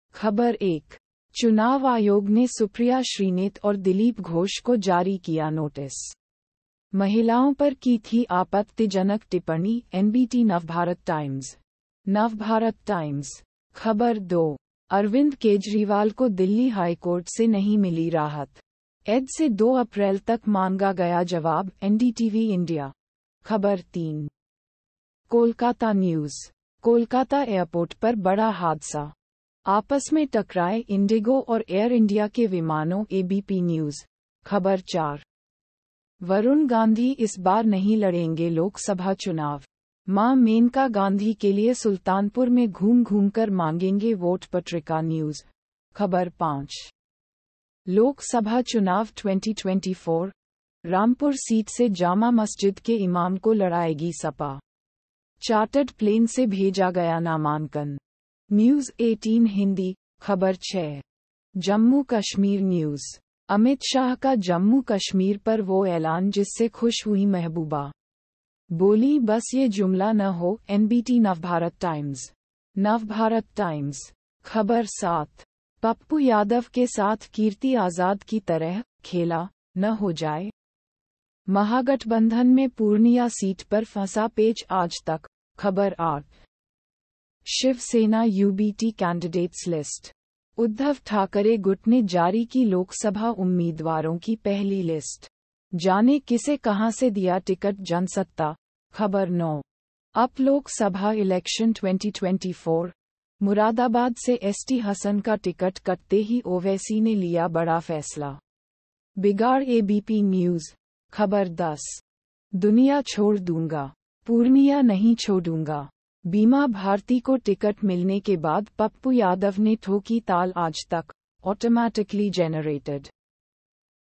Hindi News (भारत)- 27/03/2024,08:07 PM GMT+5:30
ख़बर 1:चुनाव आयोग ने सुप्रिया श्रीनेत और दिलीप घोष को जारी किया नोटिस, महिलाओं पर की थी आपत्तिजनक टिप्पणी - NBT नवभारत टाइम्स (Navbharat Times)
ख़बर 2:अरविंद केजरीवाल को दिल्ली हाईकोर्ट से नहीं मिली राहत, ED से 2 अप्रैल तक मांगा गया जवाब - NDTV India
ख़बर 3:Kolkata News: कोलकाता एयरपोर्ट पर बड़ा हादसा! आपस में टकराए इंडिगो और एयर इंडिया के विमानों - ABP न्यूज़
ख़बर 4:वरुण गांधी इस बार नहीं लड़ेंगे लोकसभा चुनाव, मां मेनका गांधी के लिए सुल्तानपुर में घूम-घूमकर मांगेंगे वोट - Patrika News
ख़बर 5:Lok Sabha Chunav 2024: रामपुर सीट से जामा मस्जिद के इमाम को लड़ाएगी सपा, चार्टर्ड प्लेन से भेजा गया नामांकन ... - News18 हिंदी
ख़बर 6:Jammu Kashmir News: अमित शाह का जम्मू कश्मीर पर वो ऐलान जिससे खुश हुईं महबूबा, बोलीं- बस ये जुमला न हो - NBT नवभारत टाइम्स (Navbharat Times)
ख़बर 7:पप्पू यादव के साथ कीर्ति आजाद की तरह 'खेला' न हो जाए? महागठबंधन में पूर्णिया सीट पर फंसा पेच - Aaj Tak
ख़बर 8:Shiv Sena UBT Candidates List: उद्धव ठाकरे गुट ने जारी की लोकसभा उम्मीदवारों की पहली लिस्ट, जानें किसे कहां से दिया टिकट - Jansatta
ख़बर 9:UP Lok Sabha Election 2024: मुरादाबाद से एसटी हसन का टिकट कटते ही ओवैसी ने लिया बड़ा फैसला, बिगाड़ - ABP न्यूज़
ख़बर 10:'दुनिया छोड़ दूंगा, पूर्णिया नहीं छोडूंगा', बीमा भारती को टिकट मिलने के बाद पप्पू यादव ने ठोकी ताल - Aaj Tak

Automatically Generated...